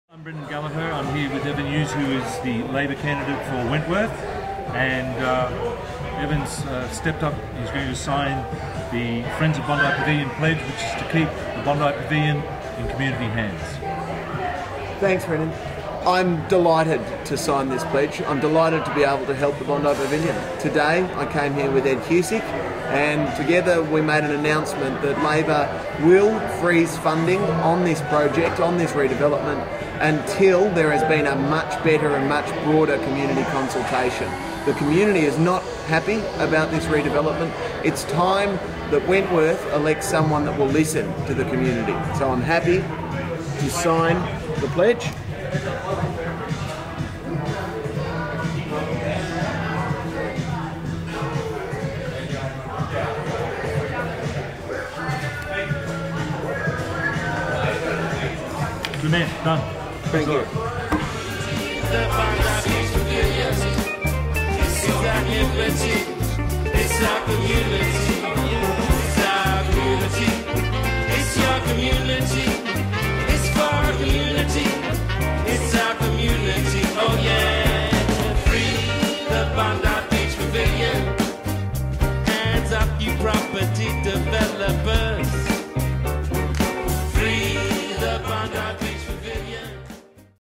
BRENDAN GALLAGHER AND EVAN HUGHES
ALP CANDIDATE FOR WENTWORTH EVAN HUGHES  SIGNS A PLEDGE TO  PRESERVE THE BONDI PAVILION FOR COMMUNITY USE.